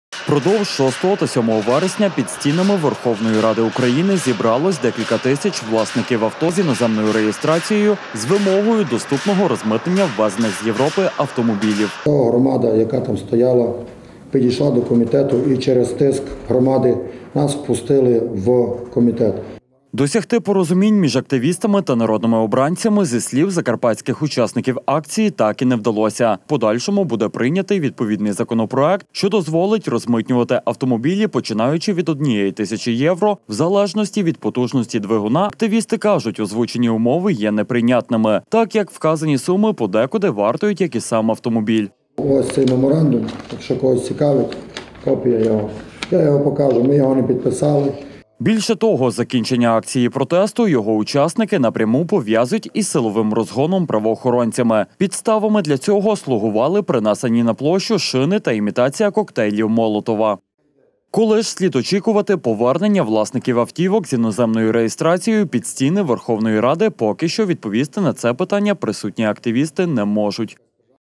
Доступне розмитнення ?
Впродовж 6 та 7 вересня під стінами Верховної Ради України зібралось декілька тисяч власників авто з іноземною реєстрацією, з вимогою доступного розмитнення ввезених з Європи автомобілів.
Досягти порозумінь між активістами та народними обранцями, зі слів закарпатських учасників акції, так і не вдалося. Активісти кажуть: озвучені умови є неприйнятними, так як вказані суми подекуди коштують як й сам автомобіль.
Більше того, закінчення акції протесту його учасники напряму пов’язують із силовим розгоном правоохоронцями. Підставами для цього слугували принесені на площу шини та імітація коктейлів молотова.
Коли ж слід очікувати повернення власників автівок з іноземною реєстрацією під стіни Верховної Ради, поки що відповісти на це питання присутні активісти не можуть